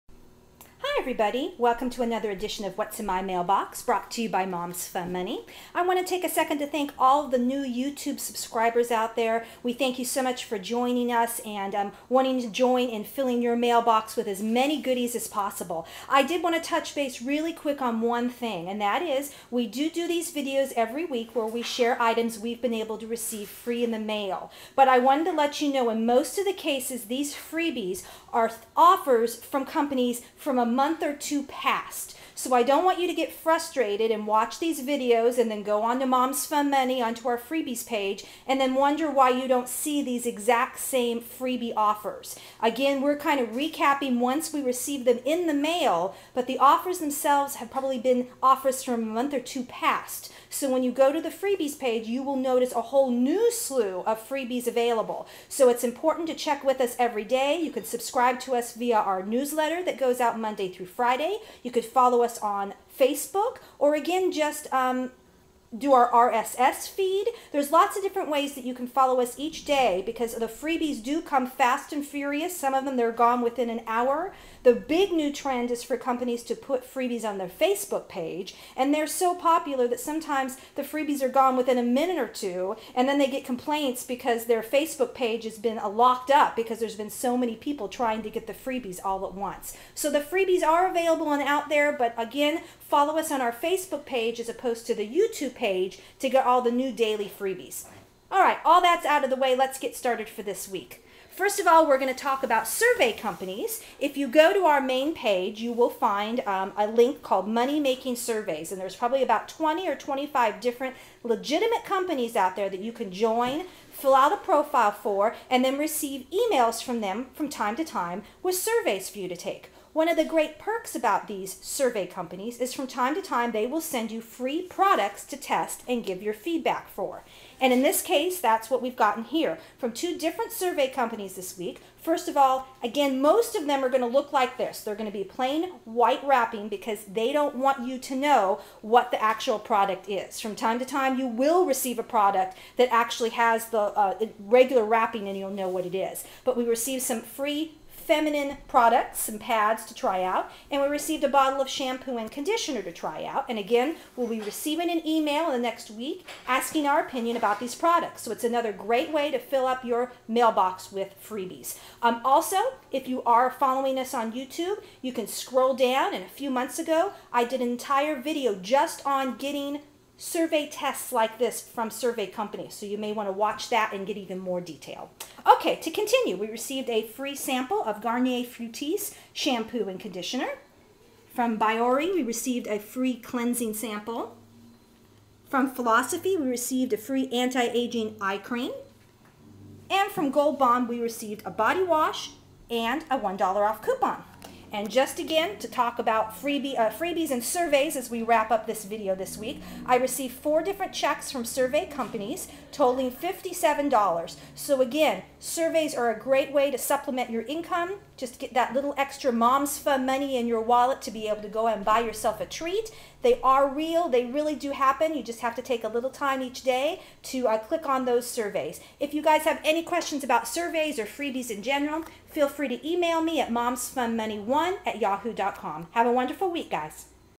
Mailbox Freebies 6/18/11
Lots of information about survey company money and product tests this week so pay attention :)